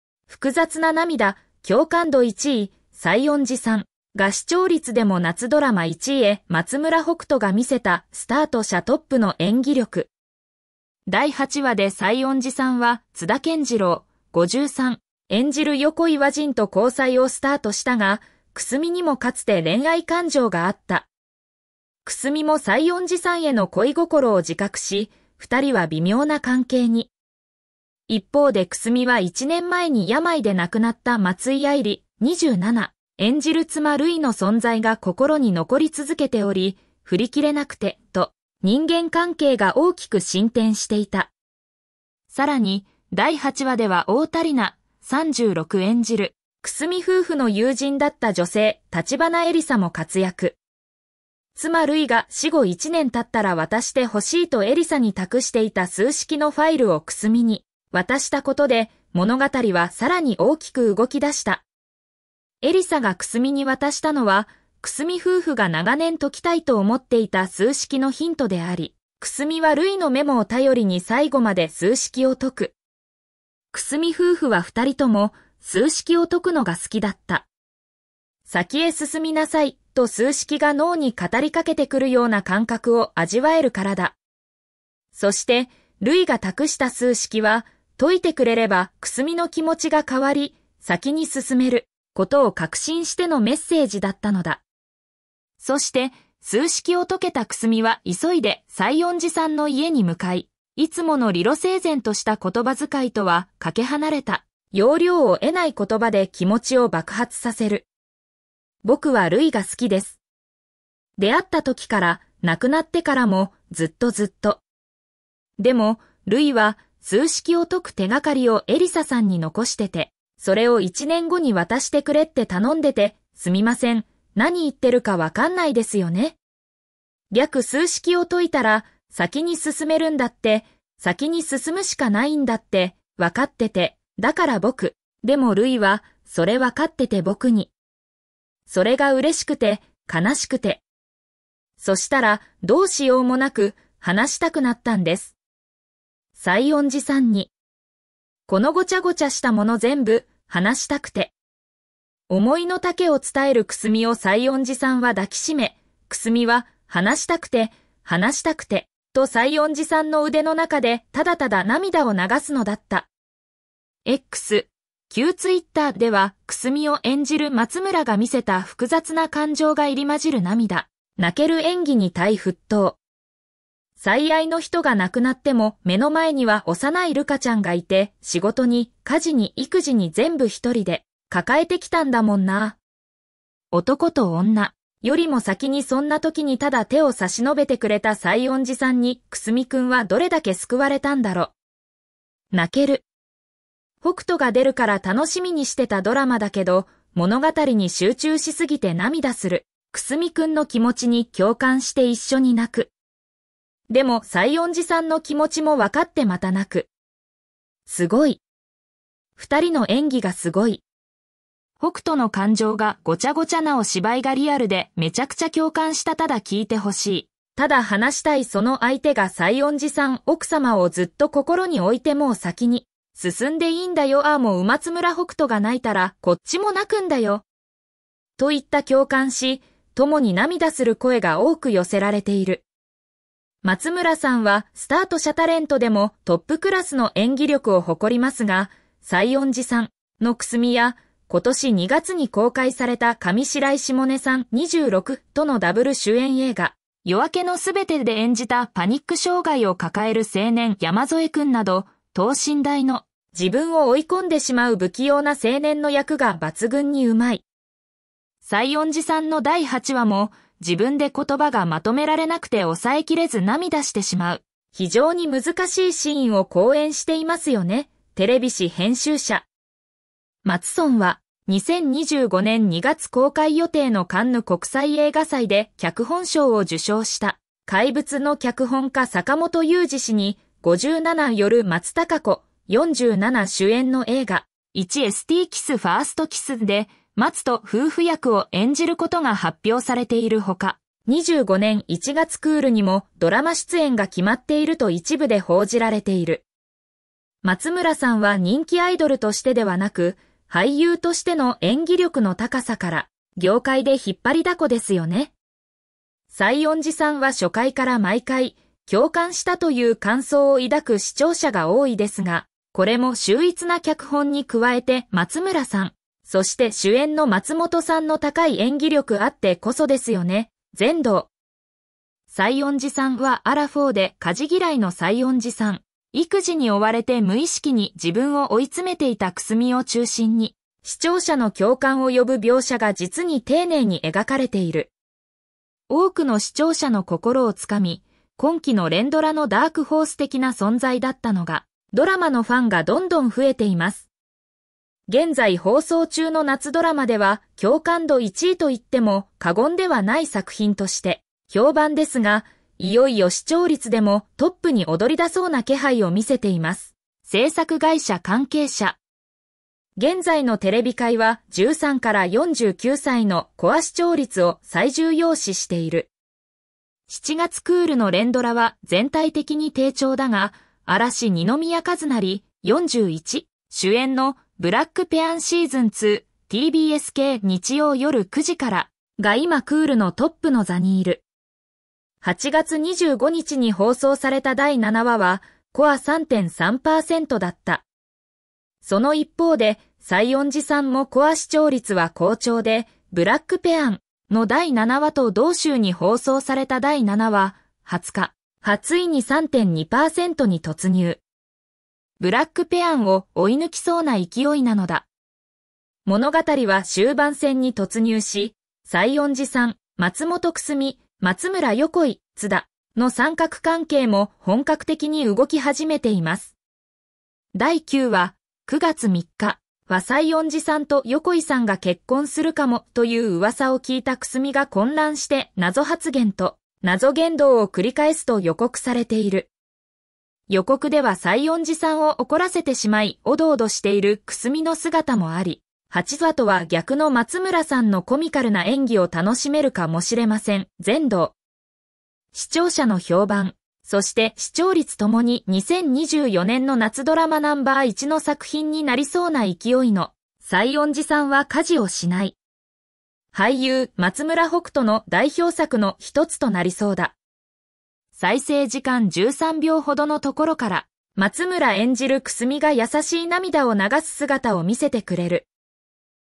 「松村北斗が魅せた『西園寺さん』共感度No.1の涙！夏ドラマ視聴率1位へ！」
松村北斗が主演する『西園寺さん』が、複雑な涙と感情を見事に表現し、共感度1位を獲得！さらに、夏ドラマとして視聴率でもトップに立つ快挙を達成。STARTO社のトップとしての演技力が話題を呼び、視聴者の心を掴んで離しません。松村北斗の演技がどのようにこの成功を生み出したのか、詳しく解説します！